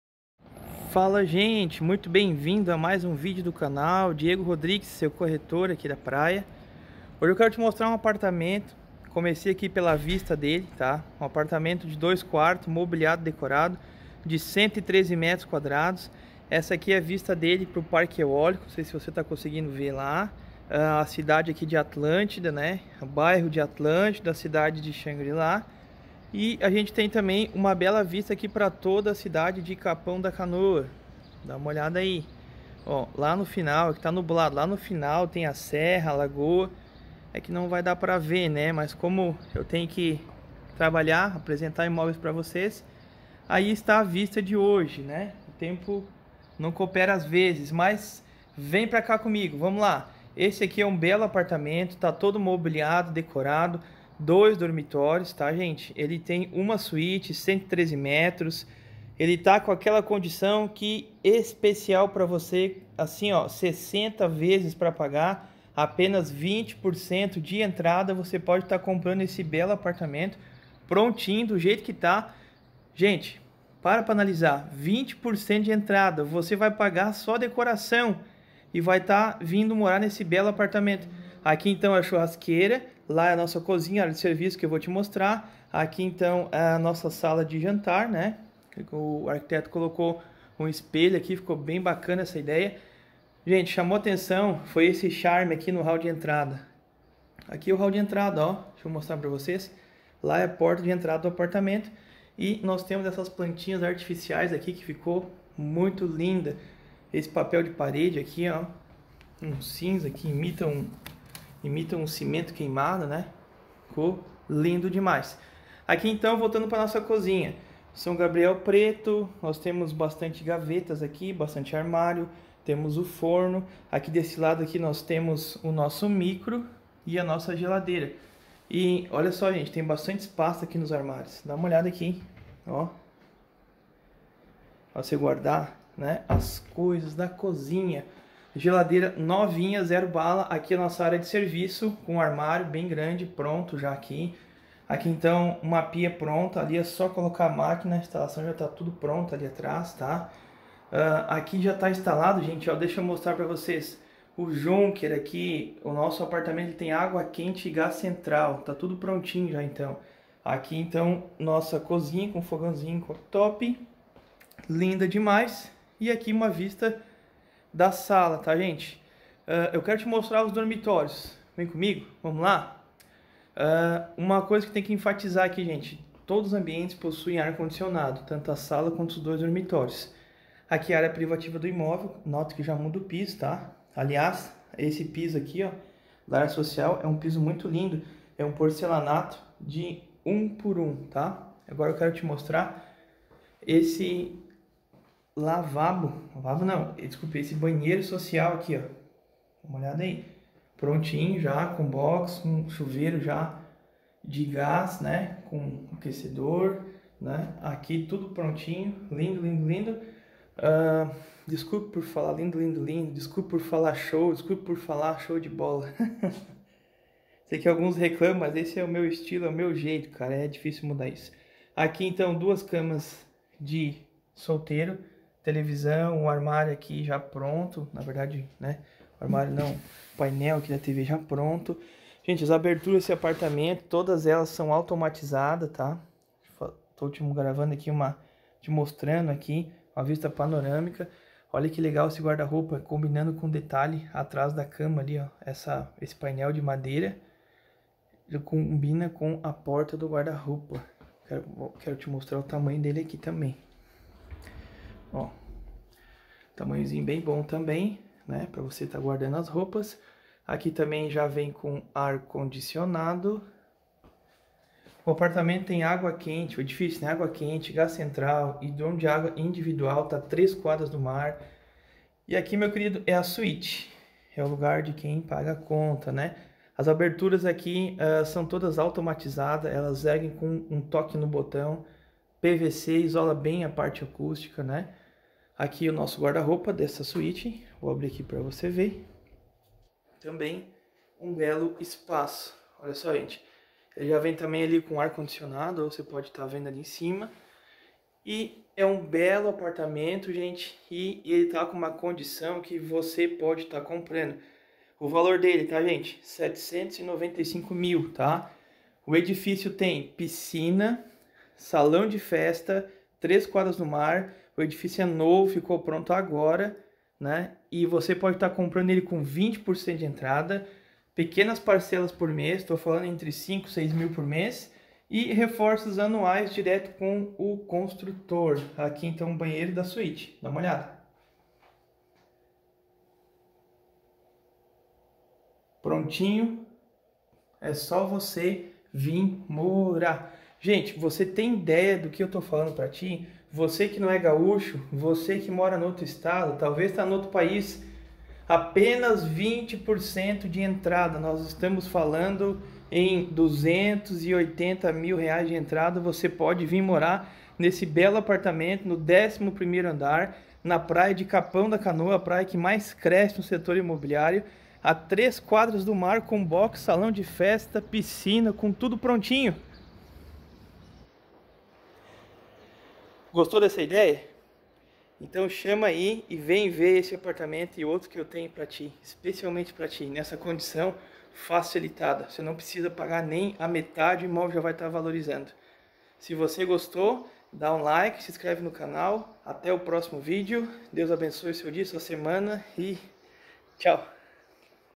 💗 AP DECORADO | COMPRE COM PEQUENA ENTRADA R$ 5 MIL POR MÊS + REFORÇOS ANUAIS 3 QUADRAS DO MAR
COMPRE ESTE BELO APE DECORADO PARCELADO DIRETAMENTE COM O PROPRIETÁRIO!

Lindo apartamento novinho nunca habitado composto de 2 dormitórios (suíte) sala de estar e jantar integrado, área de serviço, banho social, box privativo para carro, edifício com piscina, salão de festas, 2 elevadores, água quente, gás central, hidrômetro de água individual, baixo valor de condomínio, estuda receber imóvel de menor valor na praia de Capão da Canoa RS.

R$ 770 Mil

CONDIÇÃO FACILITADA:

Entrada de R$ 250 Mil + 60 x de R$ 5 Mil + 5 Reforços anuais de R$ 44 Mil
Correção INCC-M